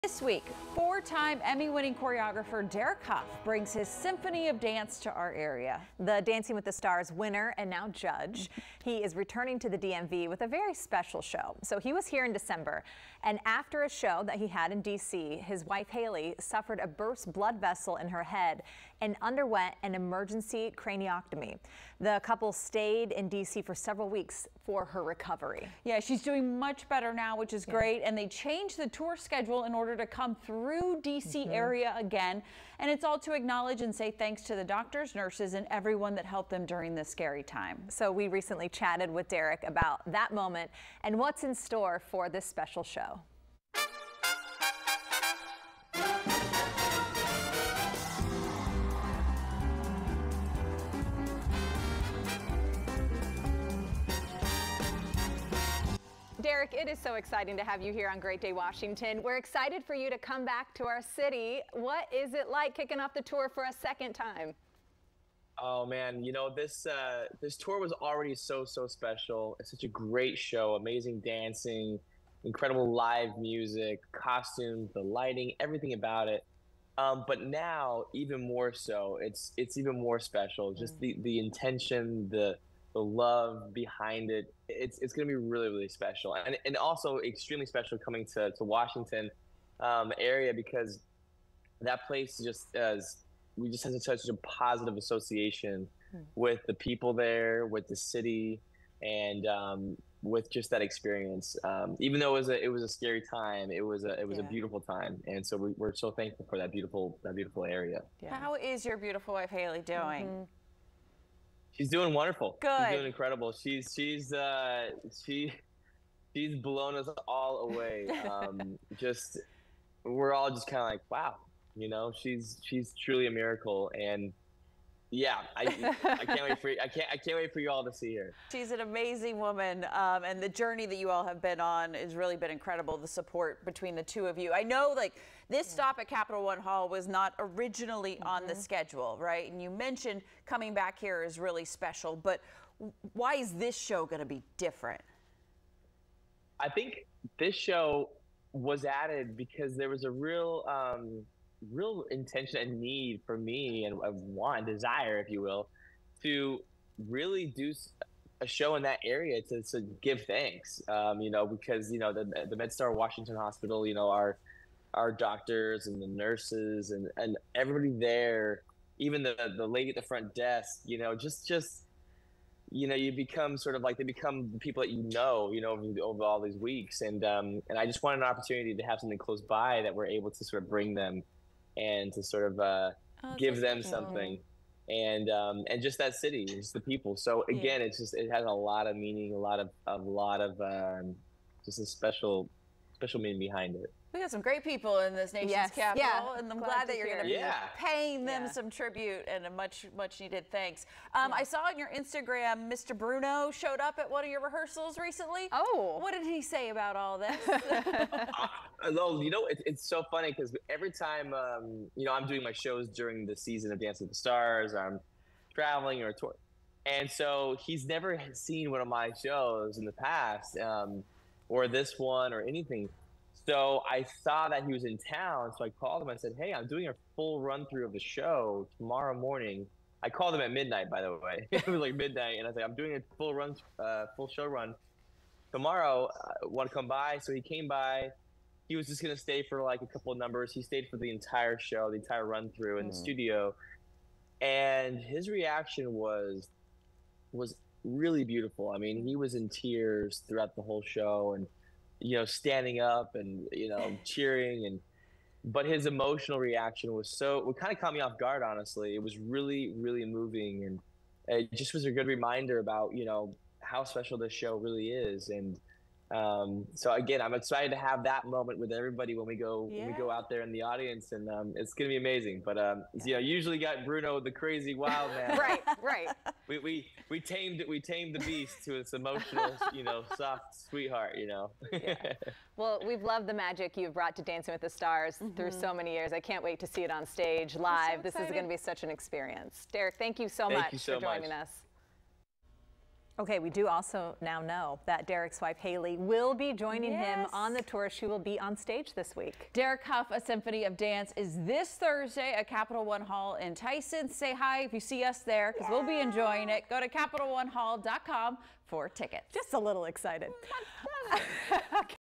4-time Emmy winner, Derek Hough returns to the DC area with his 'Symphony of Dance' tour
Emmy nominated dancer, Derek Hough, brings a special show to Capital One Hall during his 'Symphony of Dance Tour'.